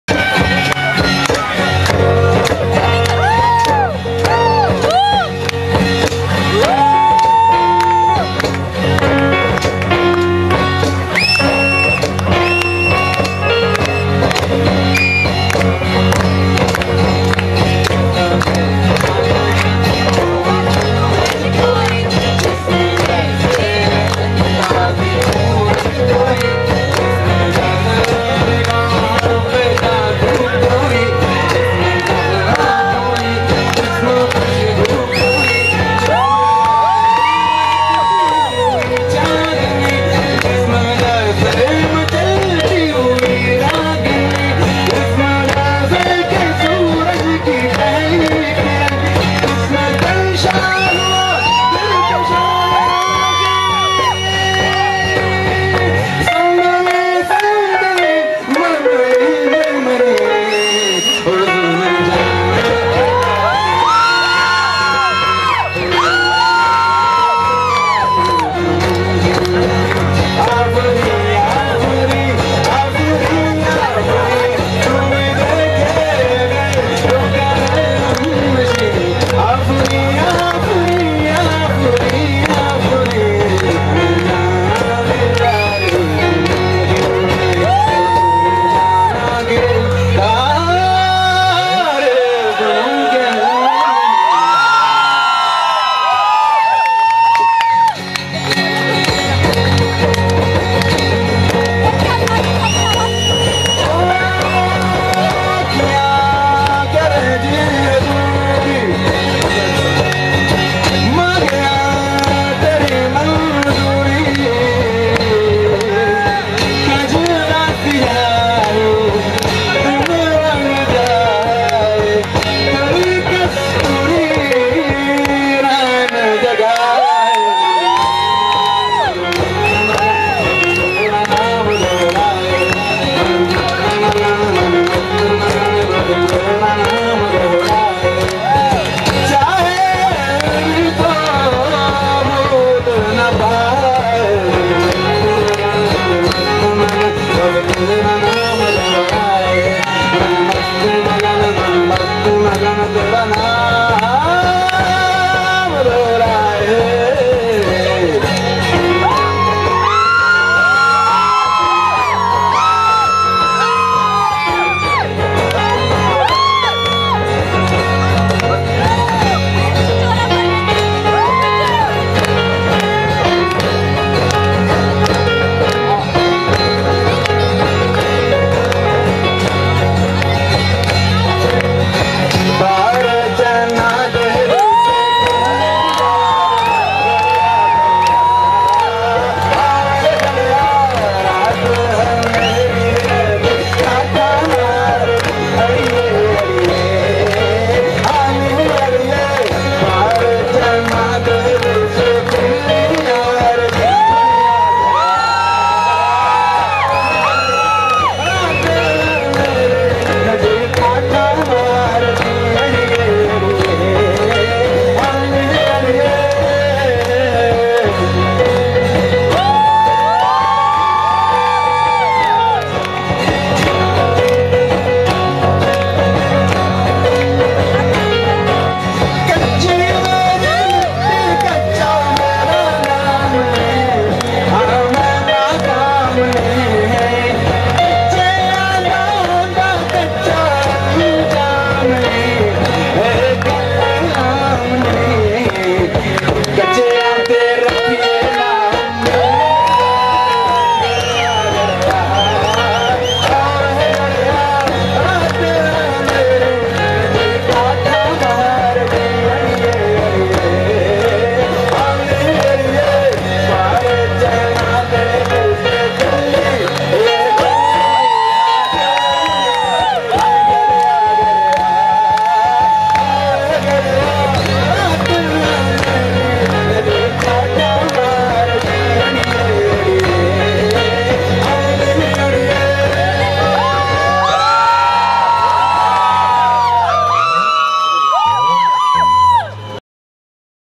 Gomal medical college ( funfair performance) , Mashup
Musical performance by Final year MBBS students of Gomal Medical College.
Dr Tanzeel ( guitar and vocals)
Dr qasim (electric guitar)
Dr saleem ( jumbee)
Dr zahoor ( piano,keyboard)